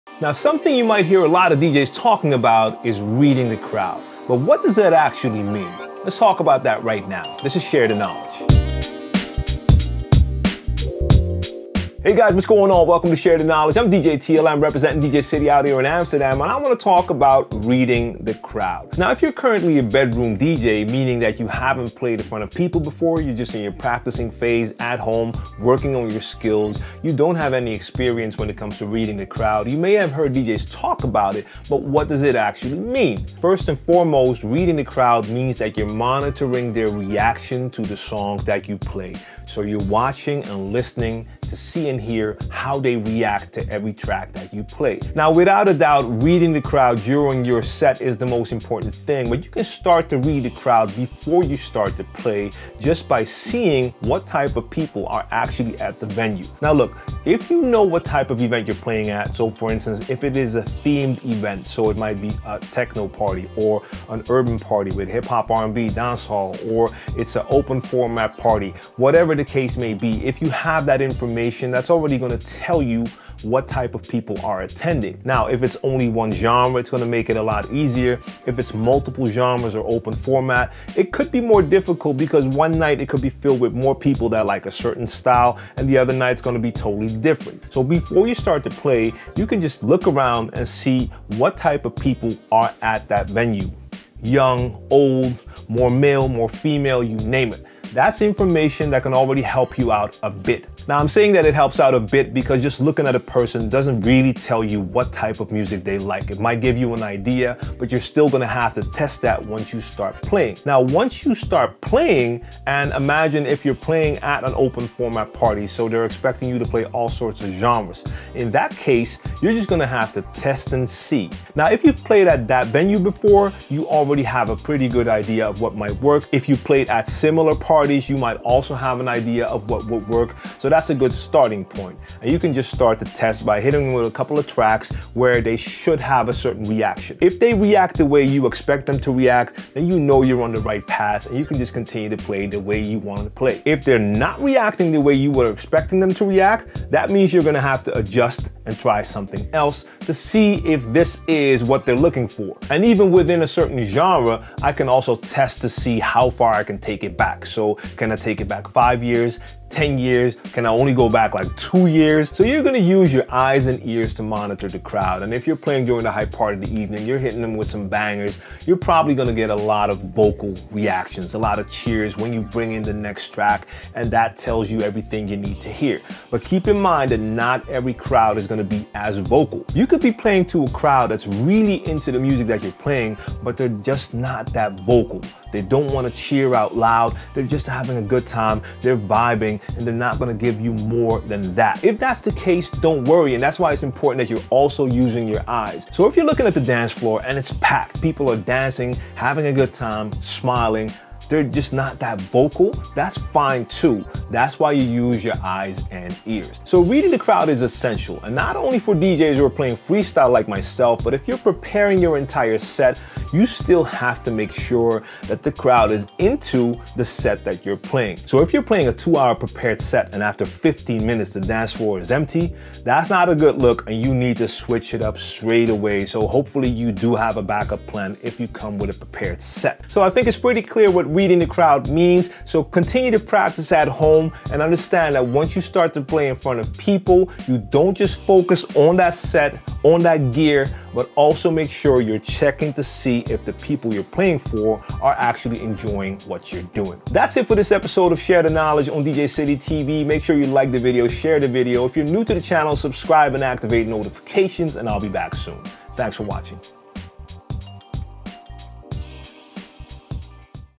How to Read the Crowd | Share the Knowledge
DJ TLM offers some tips on how to get the vibe right for your audience.